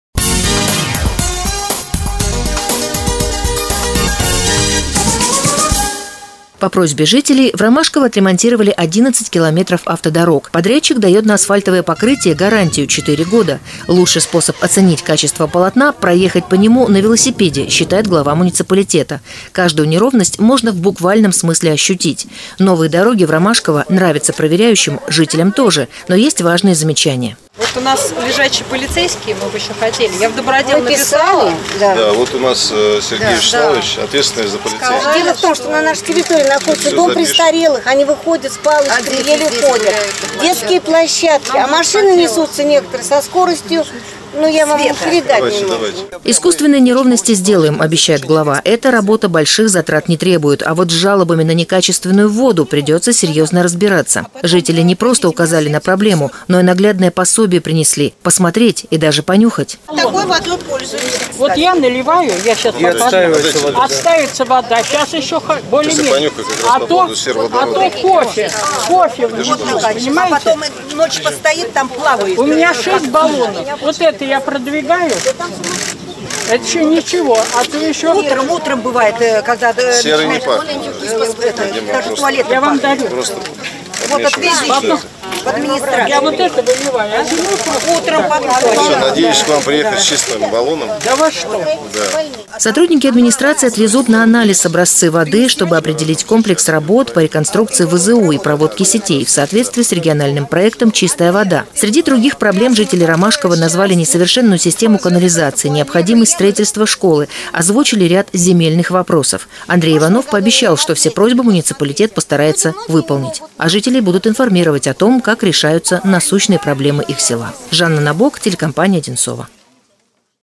Велосипедная инспекция в Ромашково
Глава Одинцовского округа Андрей Иванов в ходе велообъезда проверил качество выполненного ремонта дорожного полотна в селе Ромашково. Всего в населенном пункте отремонтировано 11 километров автодорог.